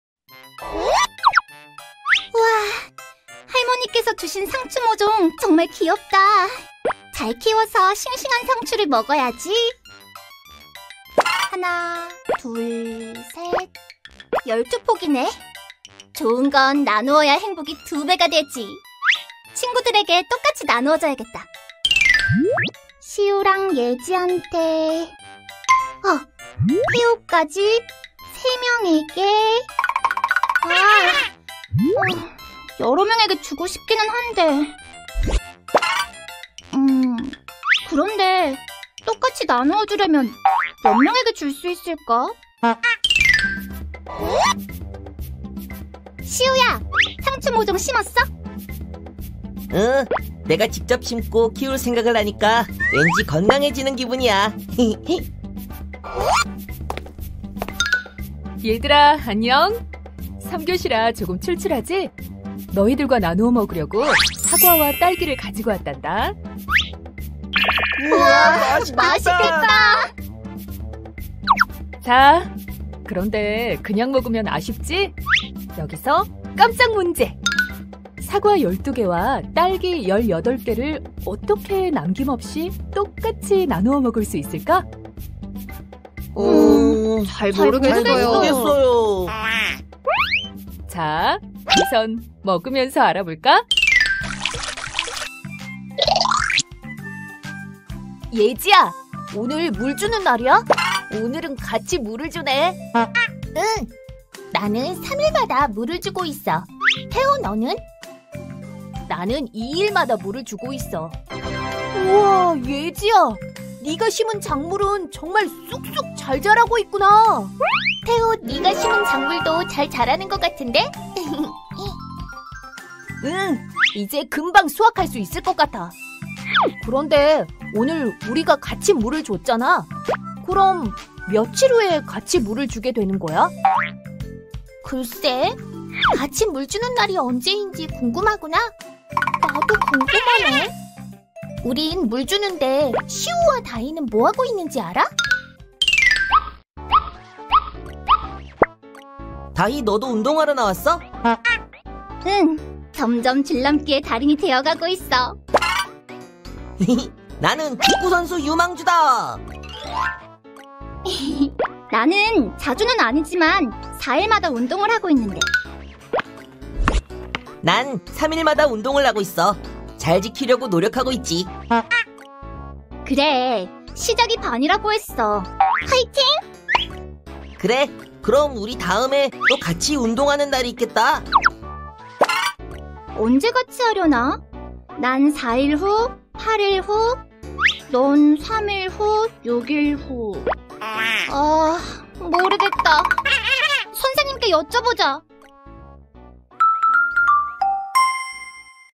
[단원 도입]5학년 1학기 2단원-약수와 배수
5학년 1학기 2단원 약수와 배수
1차시 도입을 위한 애니메이션 자료입니다.👏✨